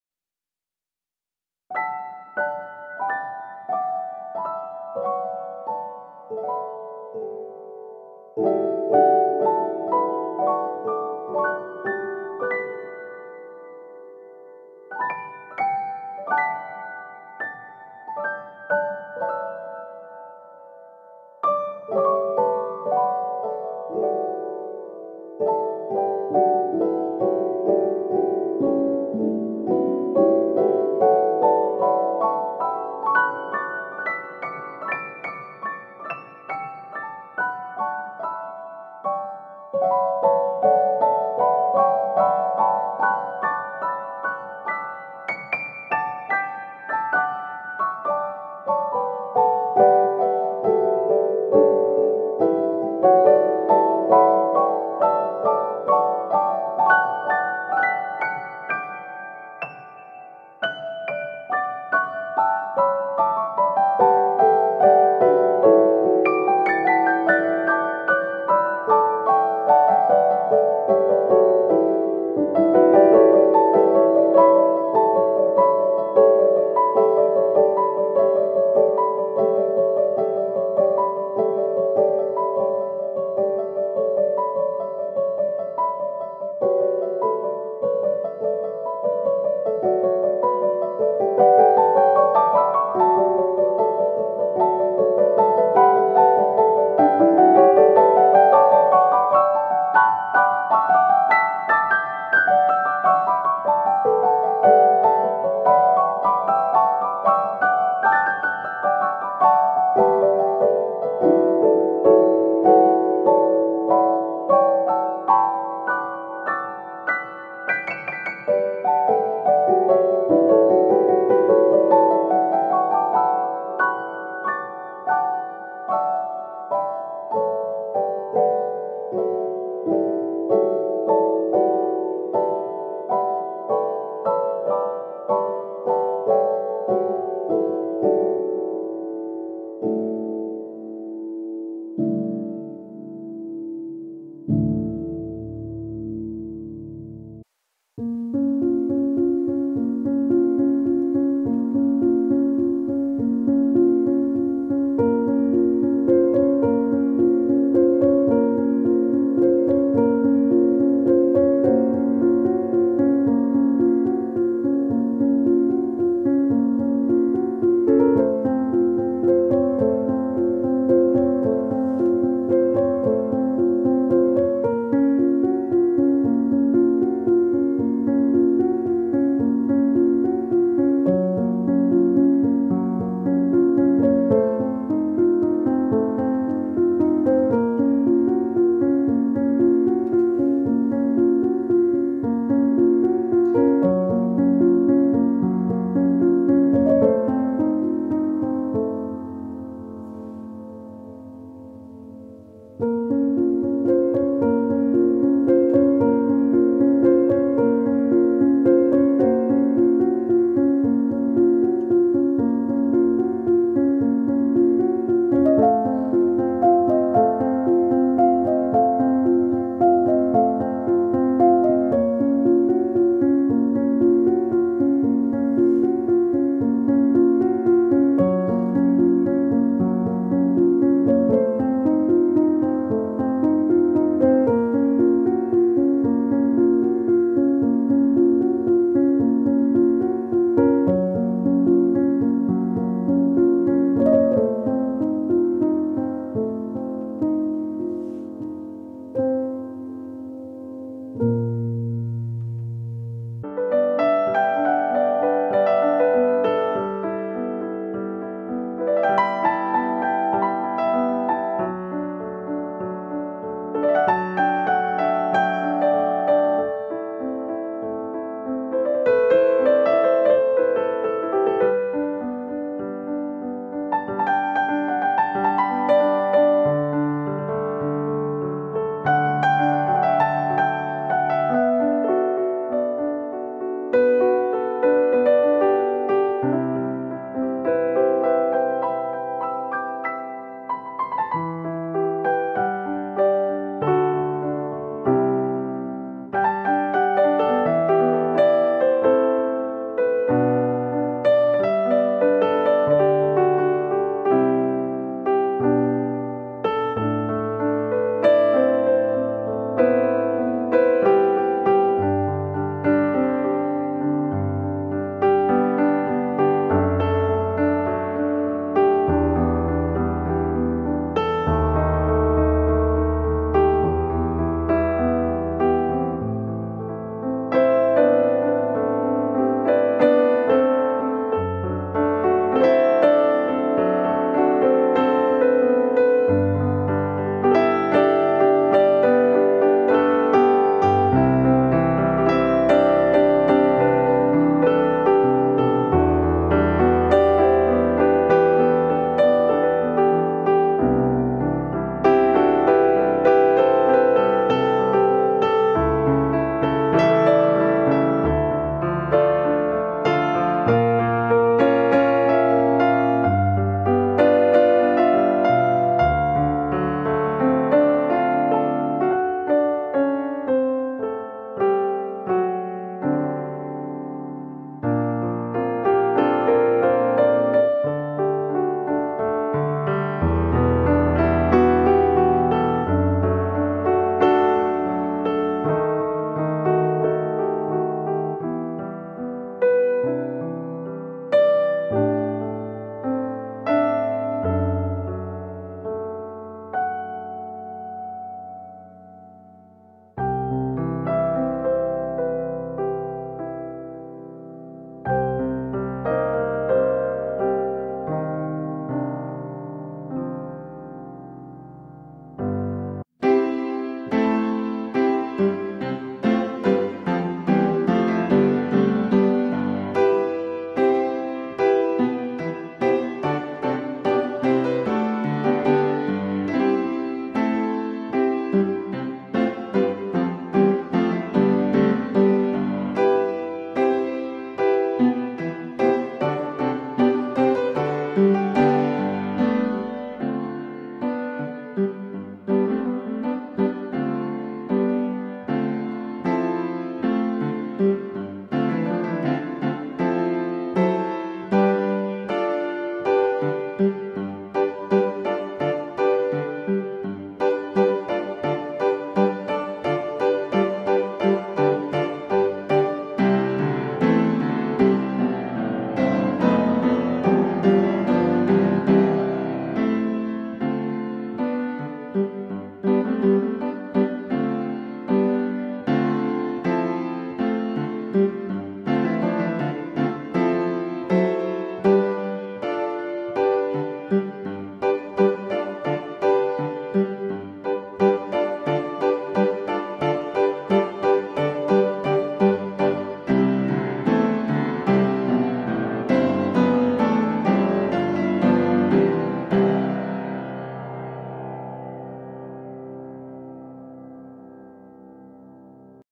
Wet Brush Relaxing Brushing Video
This video features footage of me opening my new sparkly Wet Brush, which I am now obsessed with, and brushing out my wavy hair. So, enjoy some relaxing background music with visuals of me brushing my hair.